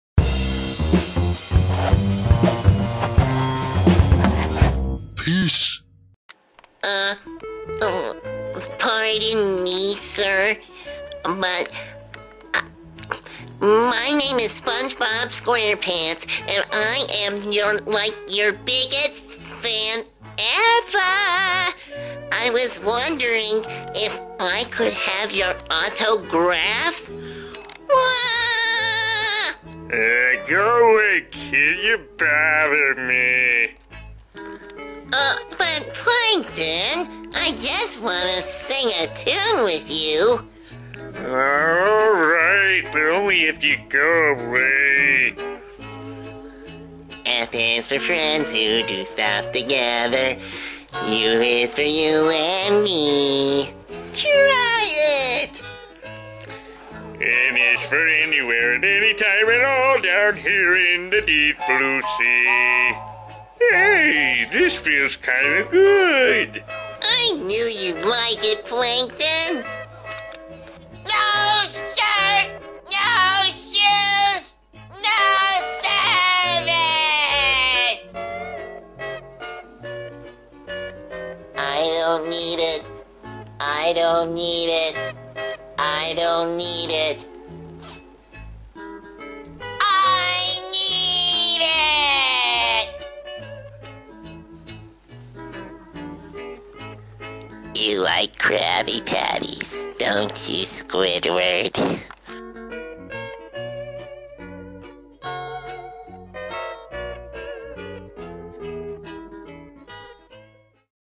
Spongebob and Plankton Impressions
Here is an impression video for you of Spongebob and Plankton. Me doing impressions of SpongeBob SquarePants. Special thanks to people like Brian Hull for sharing their wonderful talents with the world. Hope you enjoy!